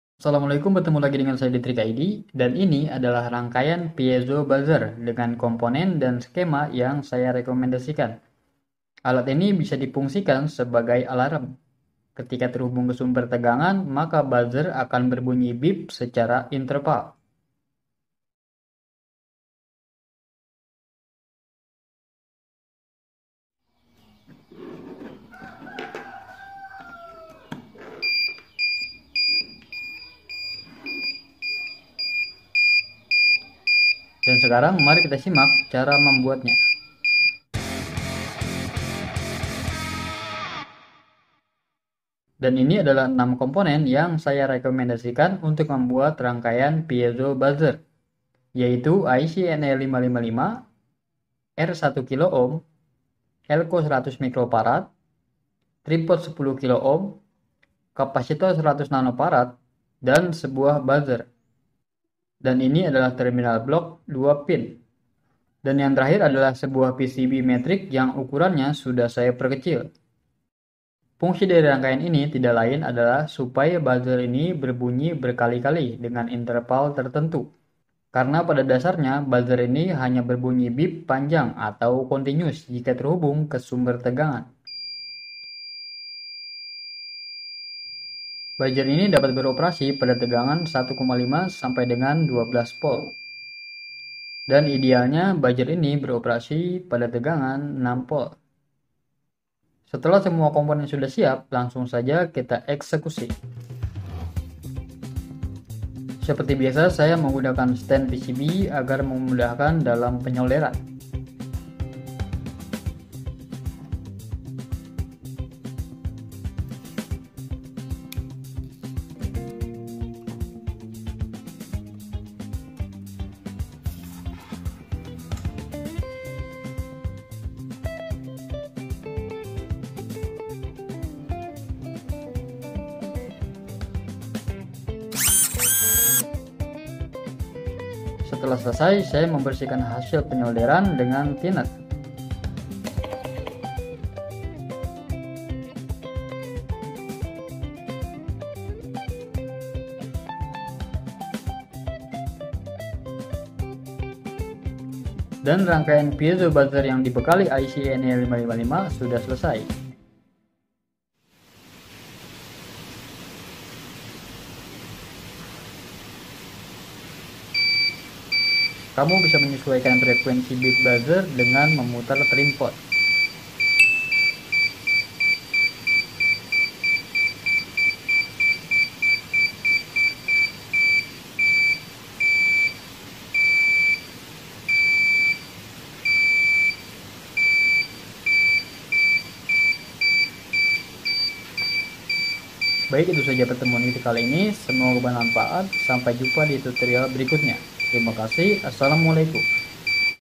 Cara Merubah Buzzer Mini Continuous Menjadi PUTUS PUTUS
The following is a simple series of PIEZO BUZZER ALARM with NE555 Mini Alarm Truk Mundur
List of beep components beep buzzer wiyh ne555 ic
1. Ne555
2. Buzzer
3. VR 103 / 10K
5. Elco 16v / 100uf
6. Terminal Block 2 PIn
7. PCB dot Matrix

This circuit can be applied for alarms